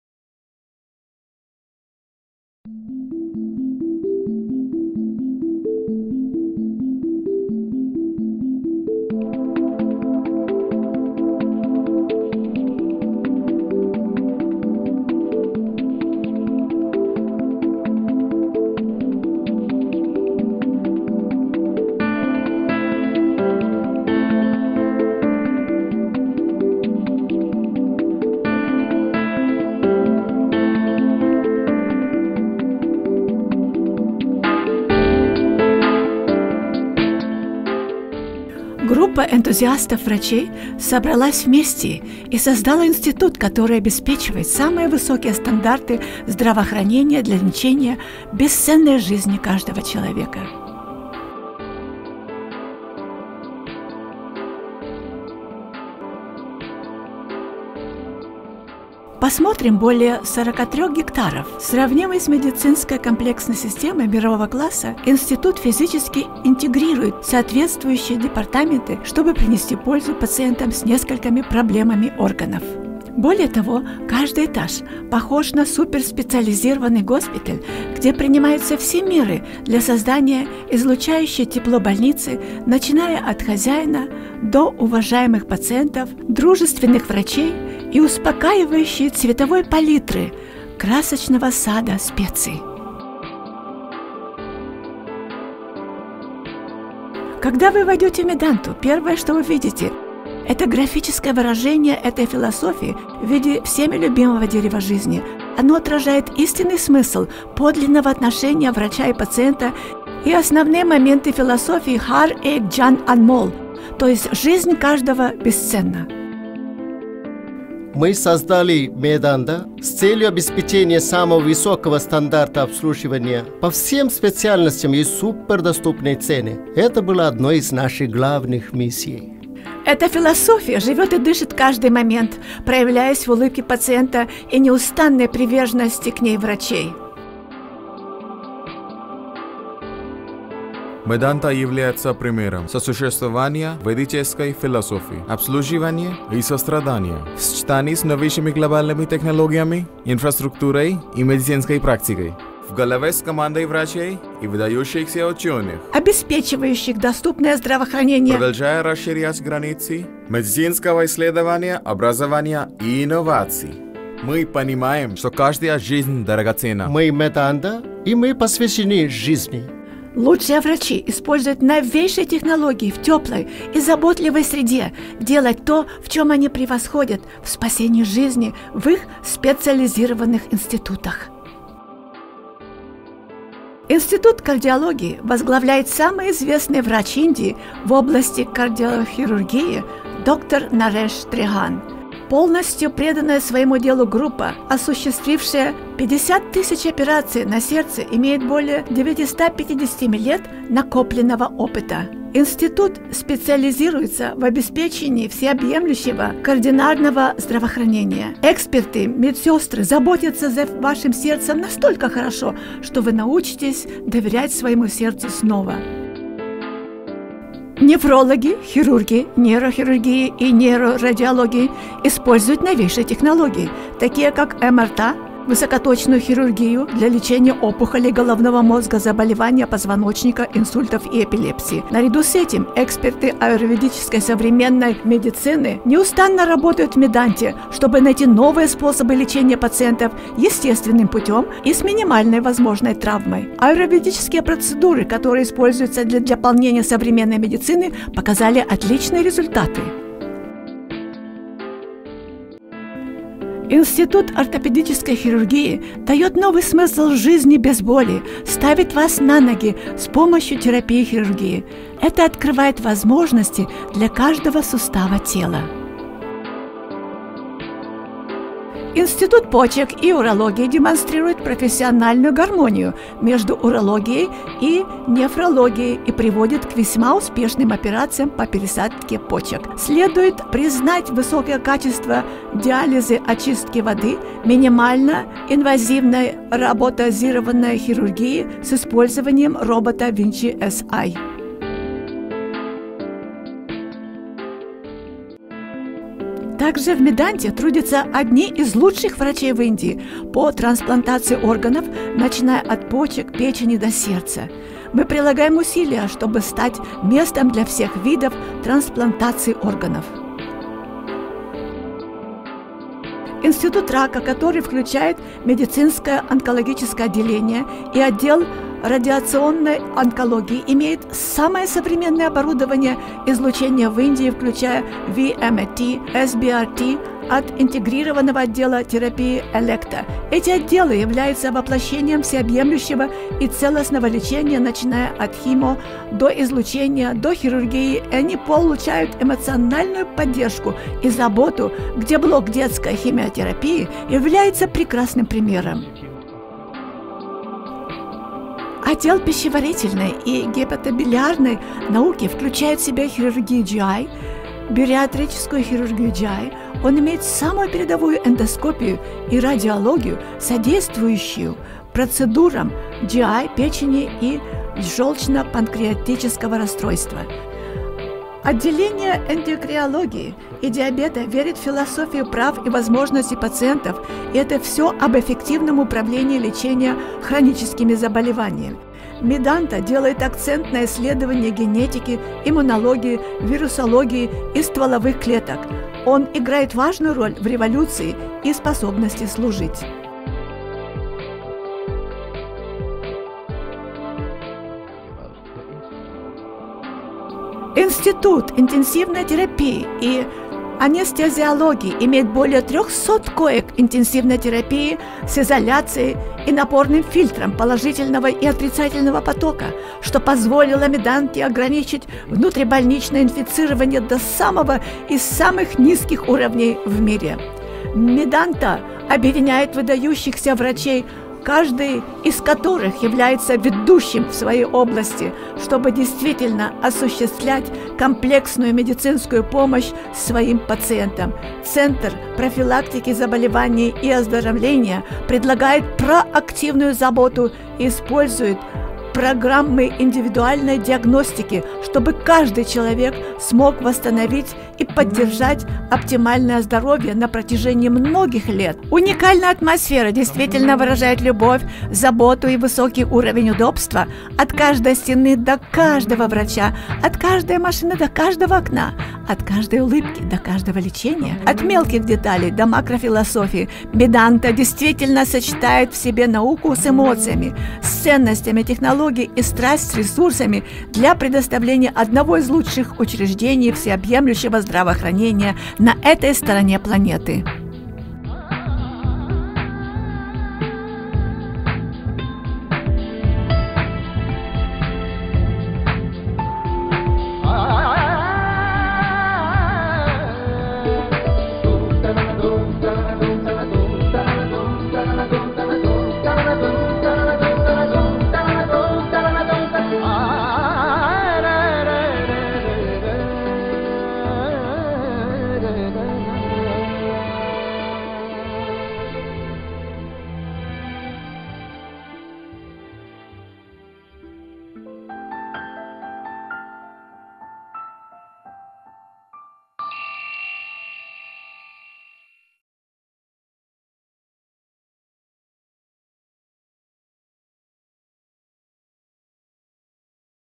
Medanta Corporate Movie-Russian
Medanta believes that the ability to heal is a boon which should be shared with everyone in need. It is known worldwide for its quality health services. Watch this corporate video in Russian to know more.